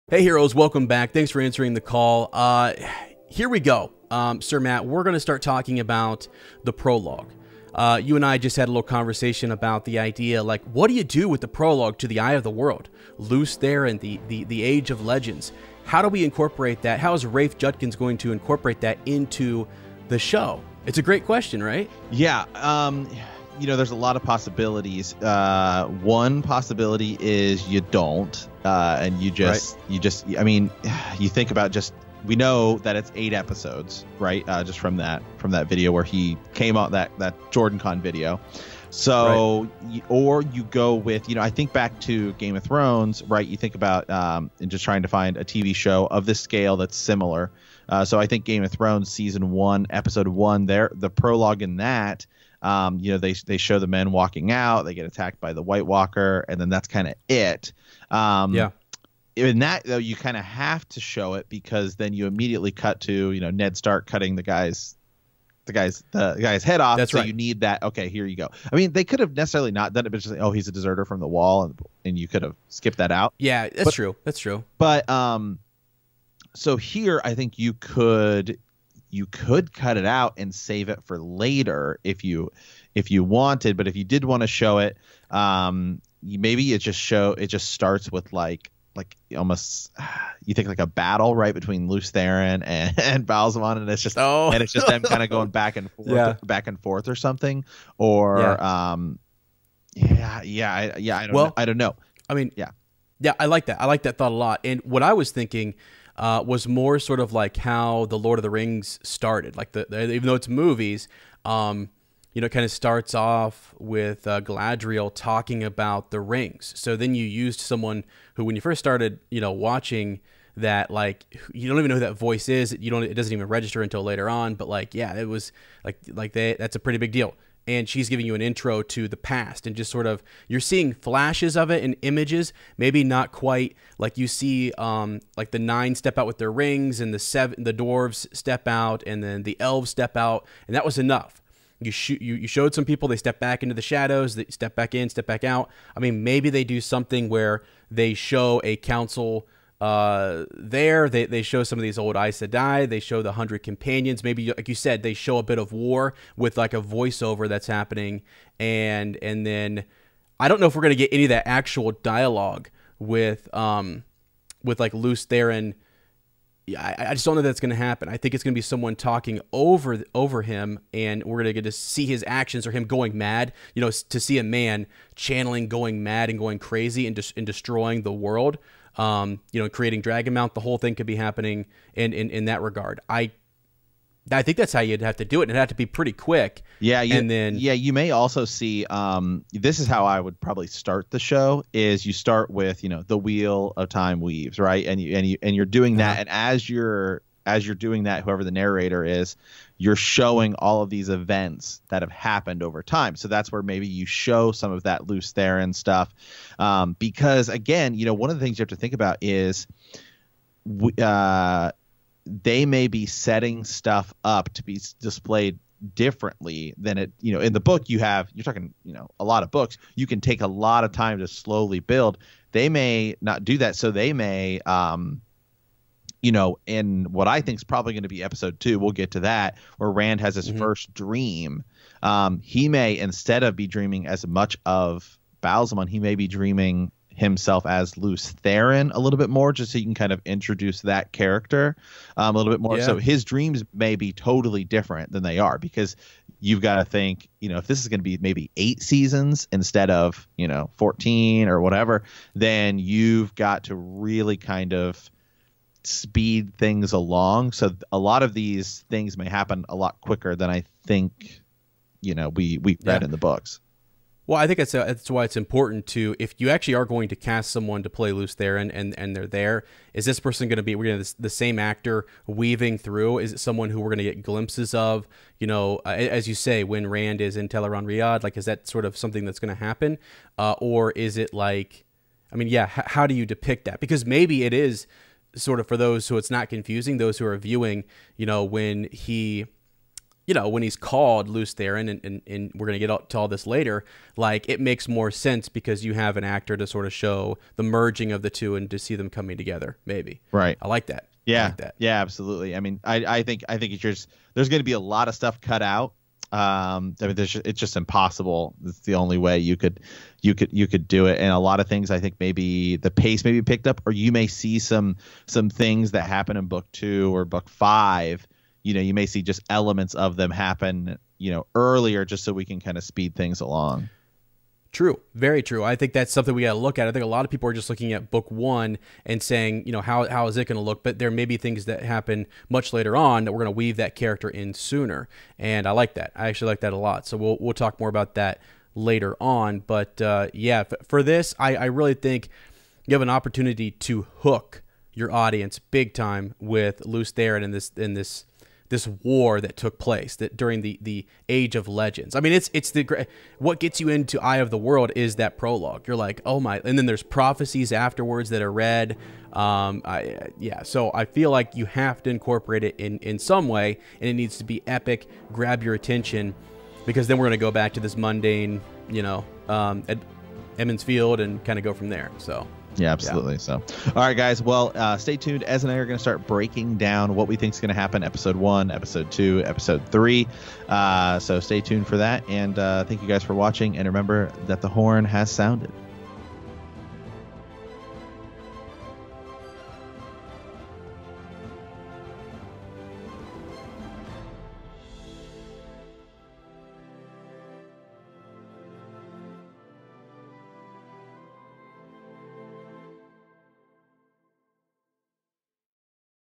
Will Lews Therin (THE DRAGON) be in Episode 1 of The Wheel of Time TV SHOW?
We know that the Dragon will ride again on the Winds of Time! How will the TV show handle the Age of Legends? How will they weave Lews Therin into Season 1?